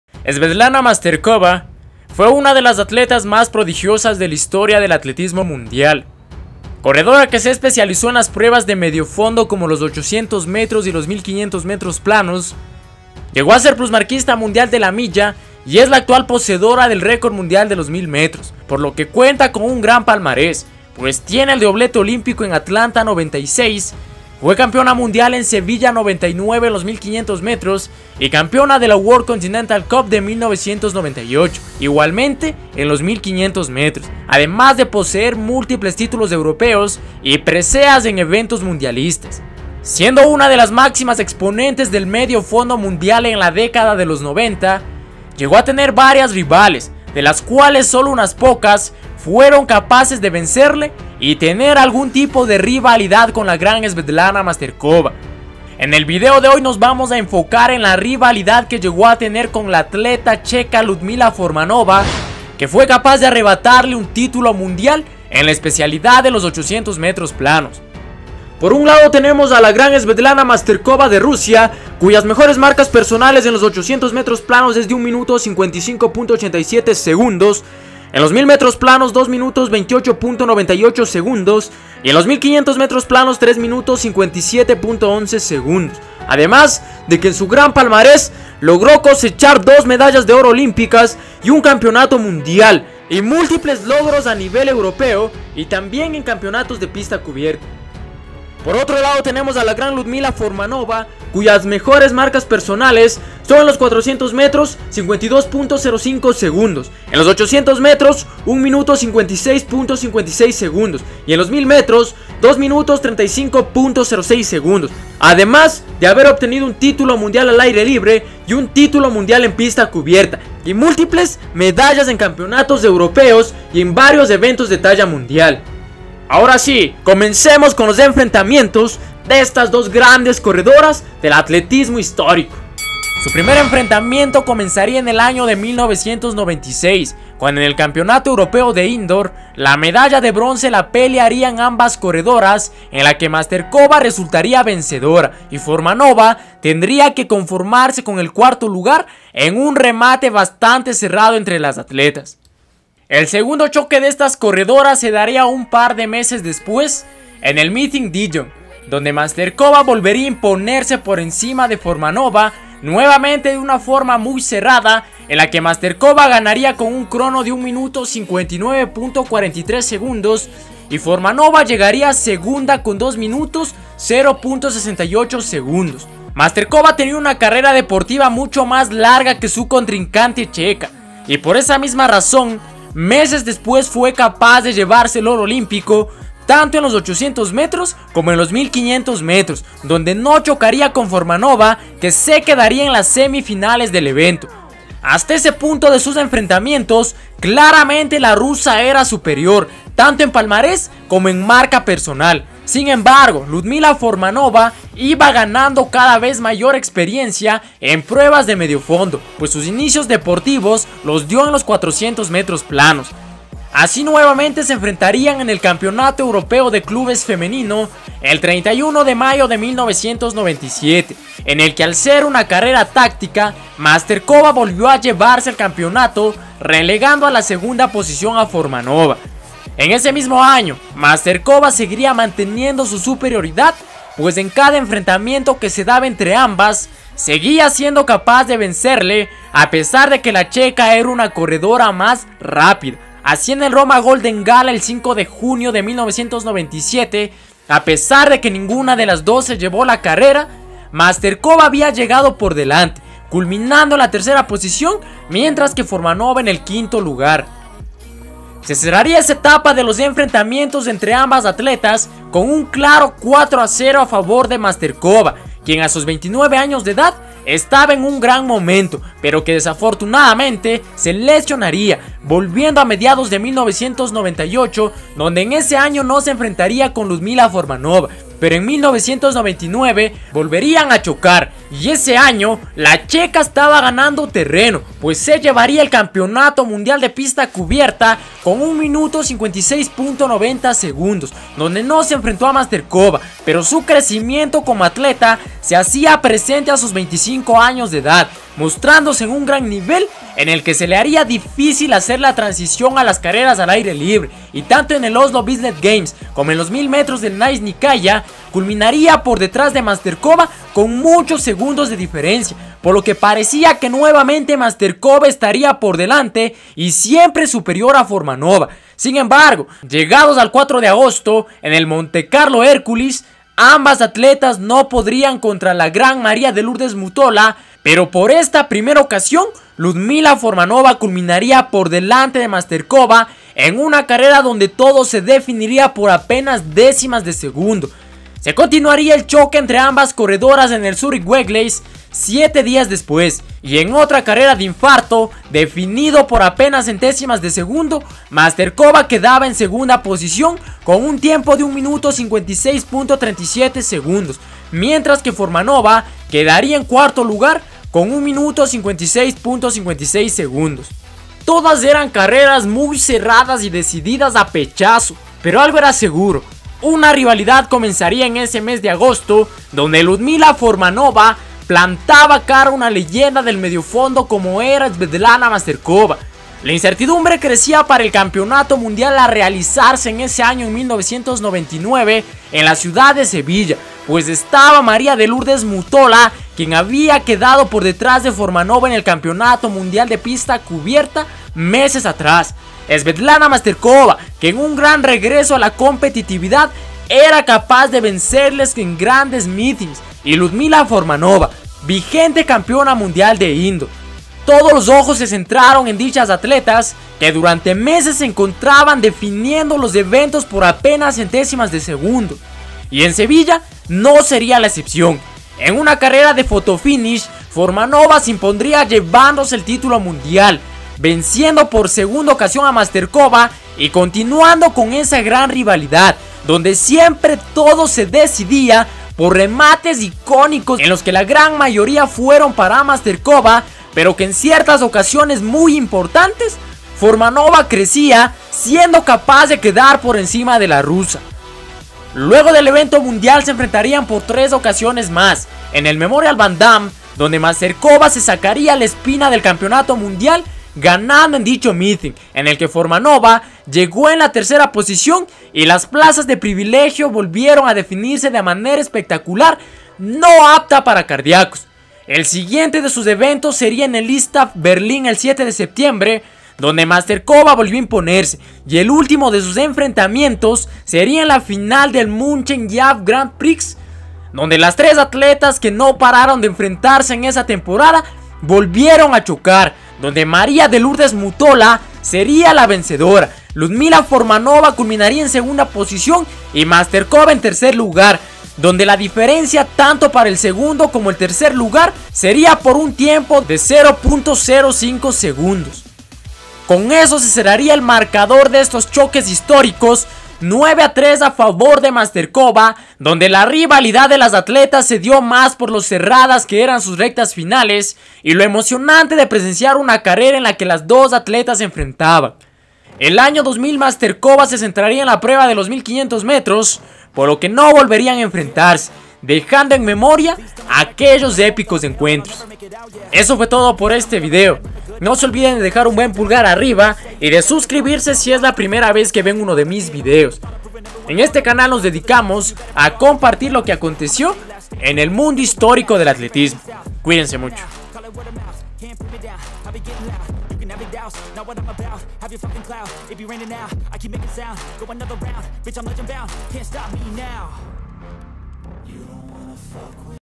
RIVALIDADES HISTÓRICAS: SVETLANA MASTERKOVA VS LUDMILA FORMANOVÁ
Saludos amigos, en este video les traigo algo un poco diferente en el que vamos a ver un recuento de una de las grandes rivalidades de la historia que llegó a tener la rusa Svetlana Masterkova en la prueba de los 800 metros.
Svetlana Masterkova, fue un atleta de las más prodigiosas de la historia del atletismo mundial, corredora que se especializó en las pruebas de medio fondo como los 800m y 1500m, llegó a ser plusmarquista mundial de la milla y es la actual poseedora del record mundial de los 1000m, por lo que cuenta con un gran palmarés, pues tiene el doblete olímpico en atlanta 96, fue campeona mundial en Sevilla 99 en los 1500m y campeona de la world continental cup de 1998, igualmente en los 1500m, además de multiples títulos europeos y preseas en eventos mundialistas.
Siendo una de las máximas exponentes del medio fondo mundial en la década de los 90, llegó a tener varias rivales, de las cuales solo unas pocas fueron capaces de vencerle y tener algún tipo de rivalidad con la gran Svetlana Masterkova. En el video de hoy, nos vamos a enfocar en la rivalidad que llegó a tener con la atleta checa Ludmila Formanová, que fue capaz de arrebatarle un título mundial en la especialidad de los 800m.